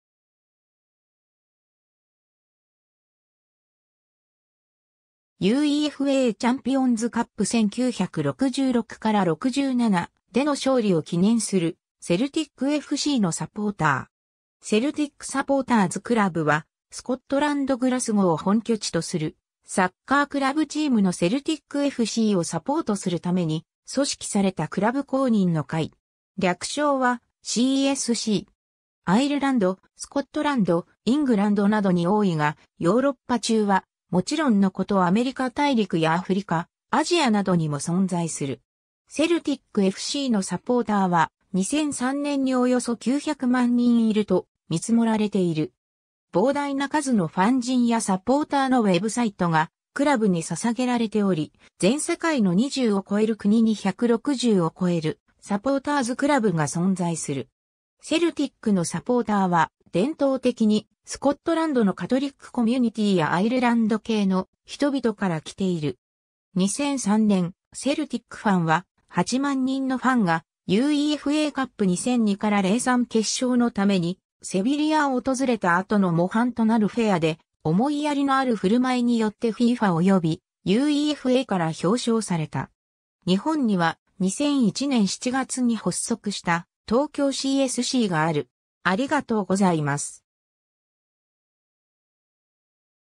セルティック・サポーターズ・クラブ
UEFAチャンピオンズカップ 1966-67での勝利を記念するセルティックFCのサポーター。
 セルティック・サポーターズ・クラブ （英: Celtic Supporters Club）はスコットランド・グラスゴーを本拠地とするサッカークラブチームのセルティックFCをサポートするために組織されたクラブ公認の会。
略称はCSC。
アイルランド、スコットランド、イングランドなどに多いが、ヨーロッパ中はもちろんのことアメリカ大陸やアフリカ、アジアなどにも存在する。
 セルティックFCのサポーターは、2003年におよそ900万人いると見積られている。
膨大な数のファンジンやサポーターのウェブサイトがクラブに捧げられており、全世界の20を越える国に160を越えるサポーターズ・クラブが存在する。
 セルティックのサポーターは伝統的にスコットランドのカトリックコミュニティーやアイルランド系の人々から来ている。
2003年、セルティックファンは、8万人のファンがUEFAカップ 2002-03 決勝のためにセビリアを訪れた後の模範となるフェアで思いやりのある振舞いによってFIFAおよびUEFAから表彰された。
 日本には、2001年7月に発足した東京CSCがある（中村俊輔が入団したのは2005年8月）。